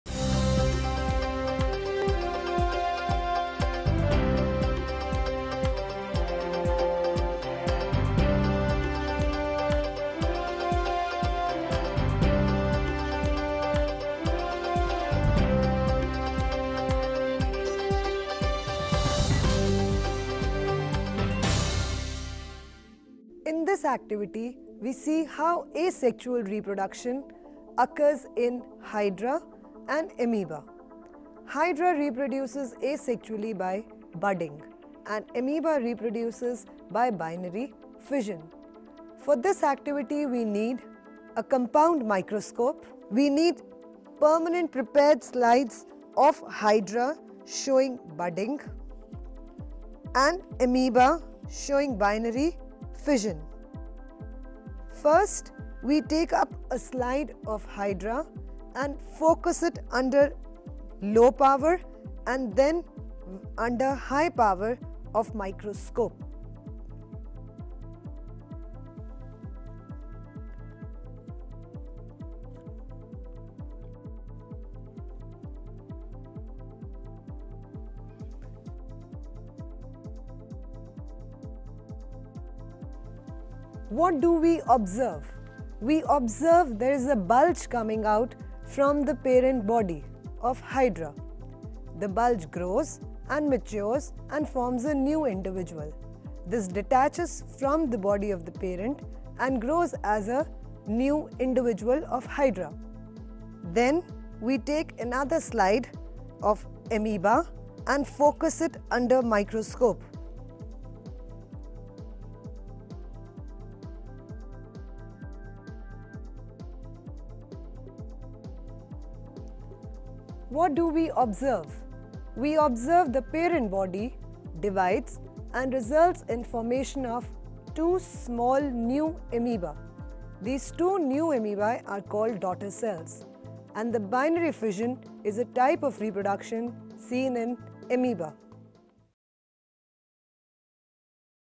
Asexual Reproduction In Hydra And Amoeba
The link enables us to understand the asexual reproduction in Hydra and Amoeba.